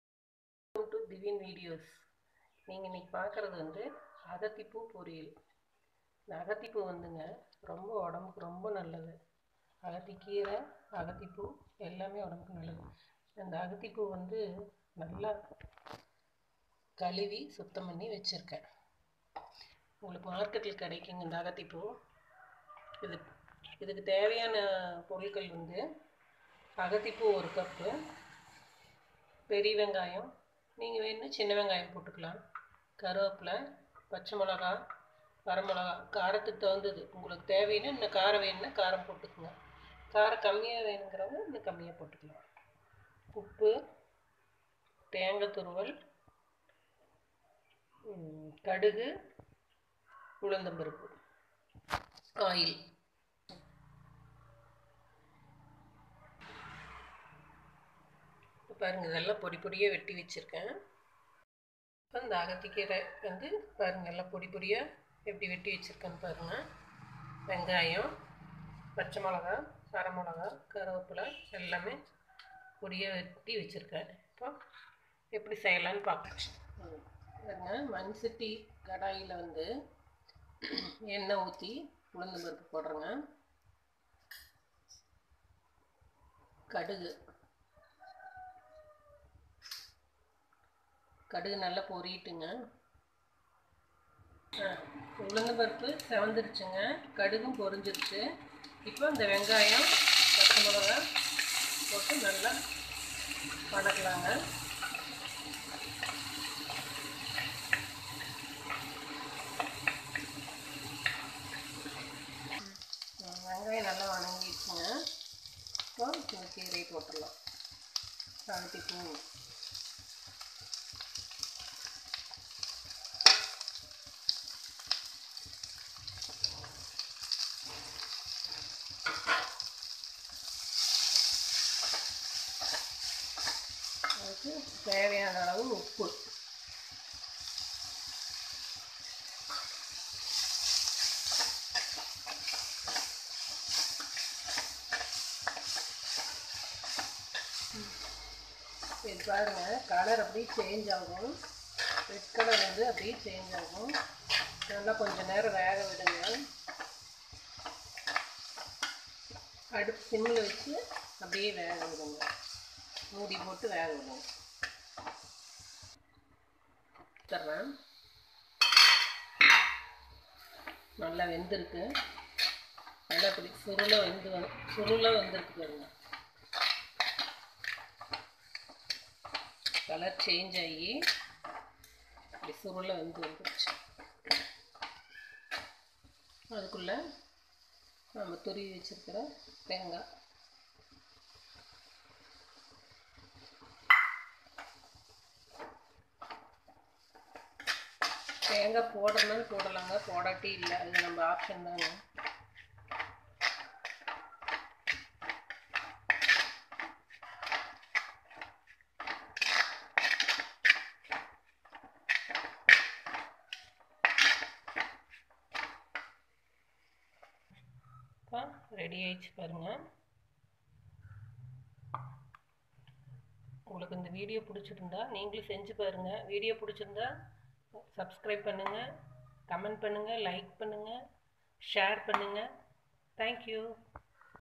Agathi flower poriyal
How to make agathi flower poriyal very easy to learn